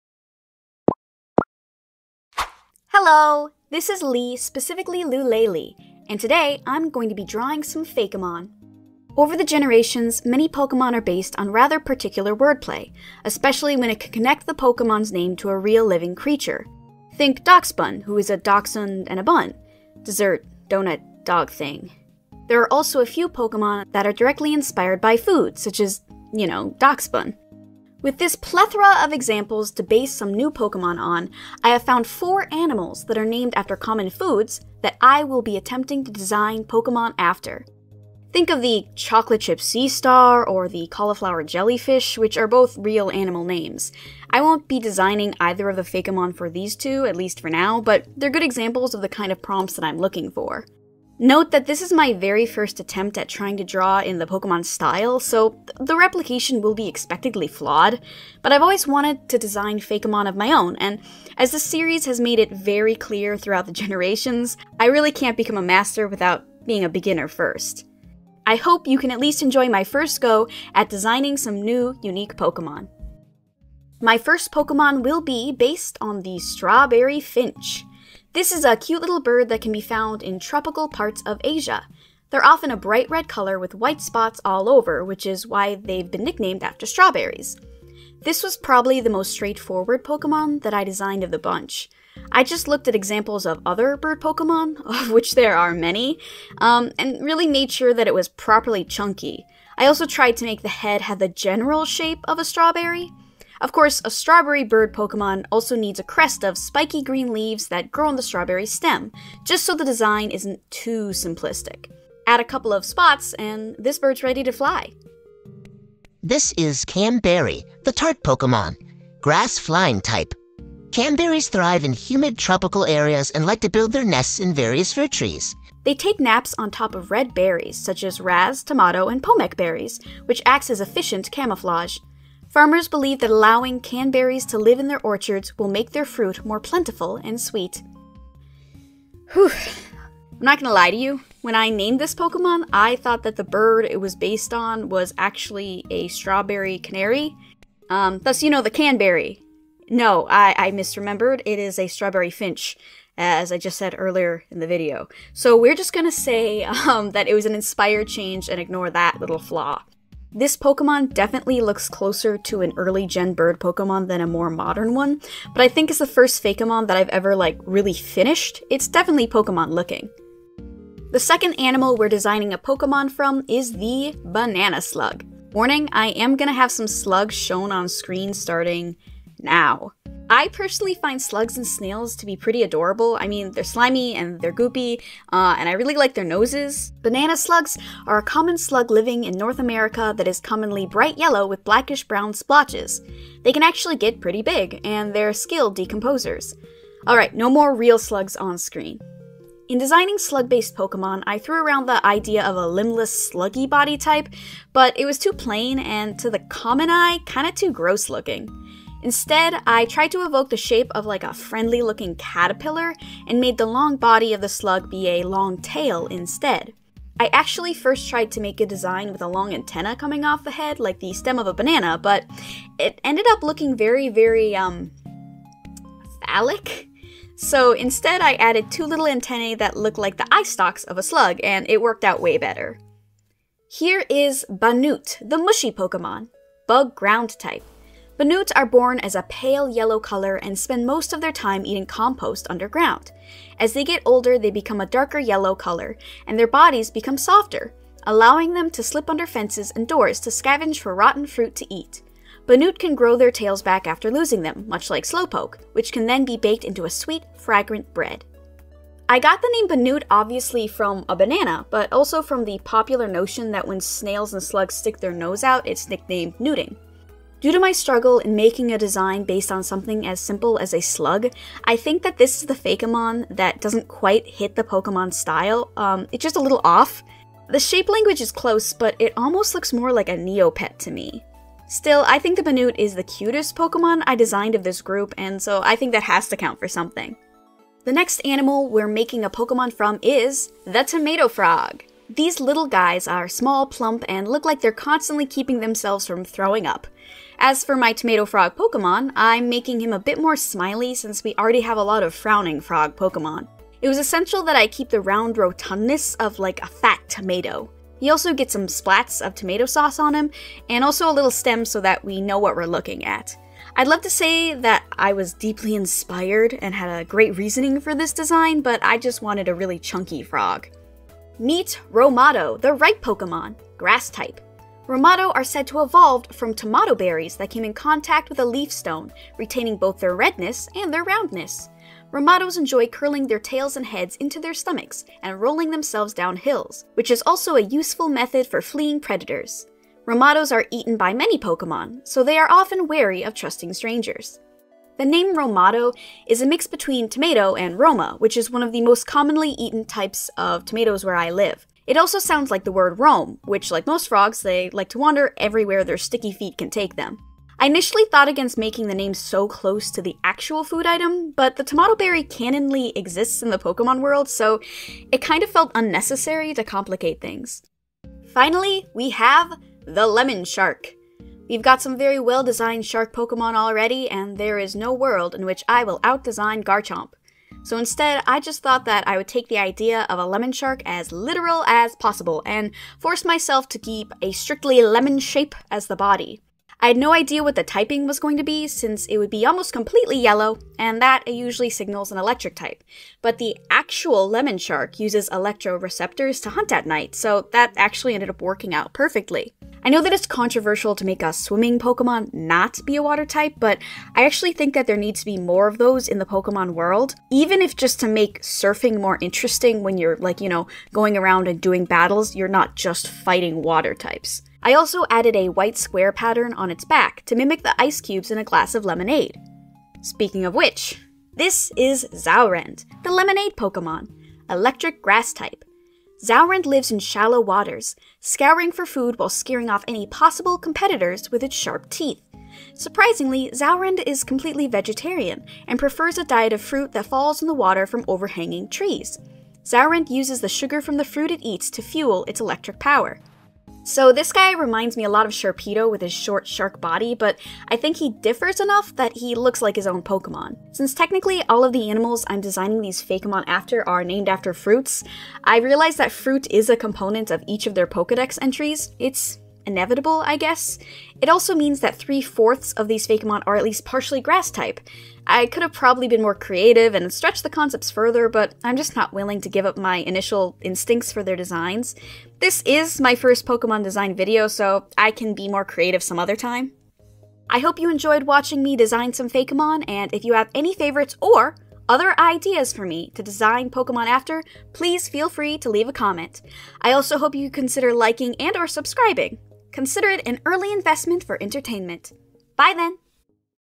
Designing Pokémon Based on Animal-Food Names
Here's my first upload! I hope someone out there enjoys it!
I'd love to make more videos like this, so if you want to see more design processes like this one, please feel free to comment any ideas you might want me to try out.

I do not own Pokémon, and all original creations in this video are of my own design.